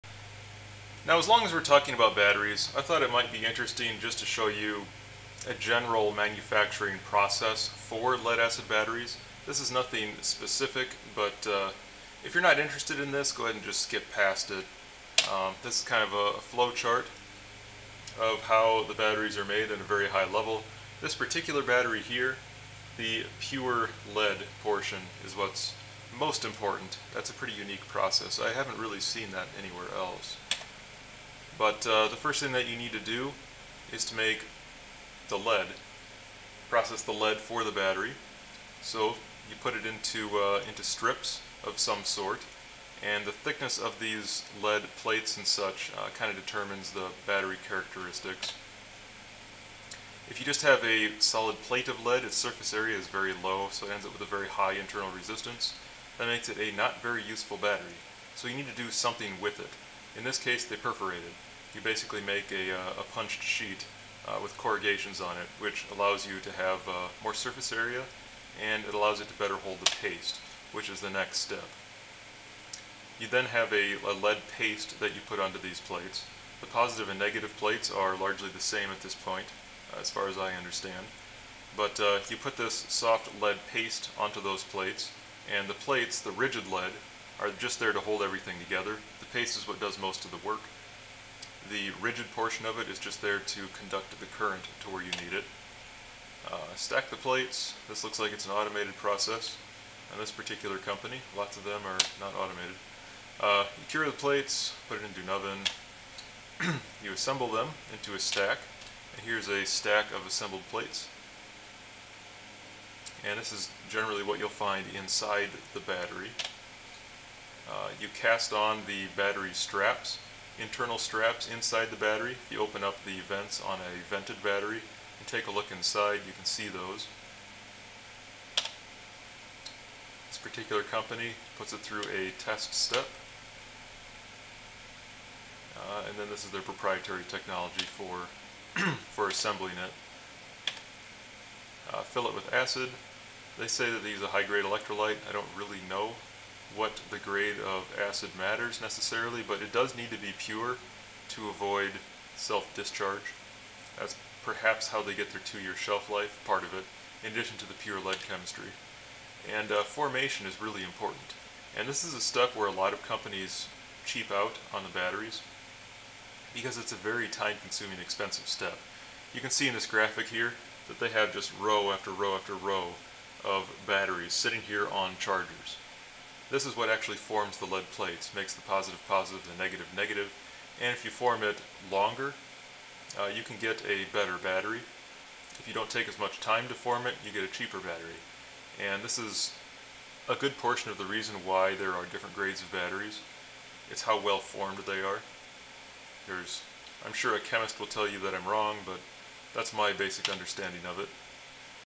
1000Ah Battery Bank Part2 - Quick Overview of a Battery Production Line
I thought it might be interesting to show a very high level overview of how batteries are made.  My power backup project will continue in part3.